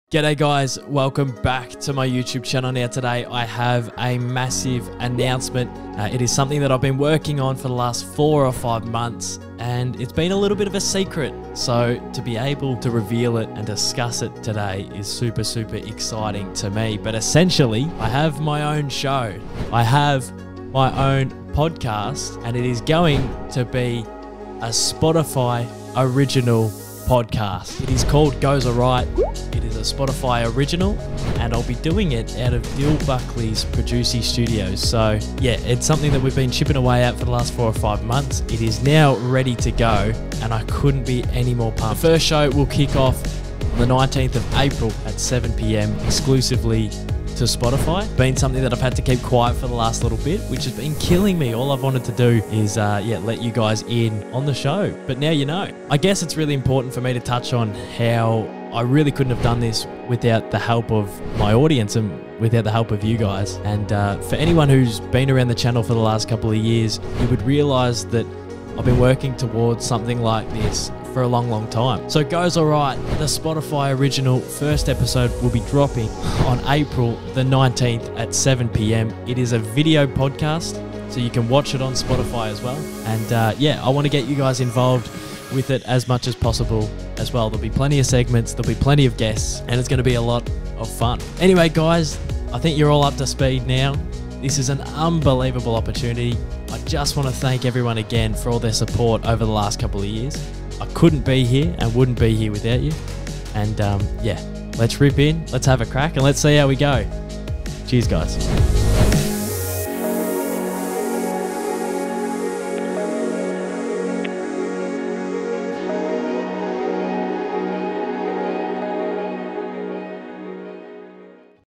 I GOT MY DREAM JOB! (Spotify Original Podcast)
Brand New Podcast 'Goes Alright' Starting on Spotify, April 19th.

Super excited to bring this show to life, so appreciative of the support I've received over the past few years, this is just the beginning!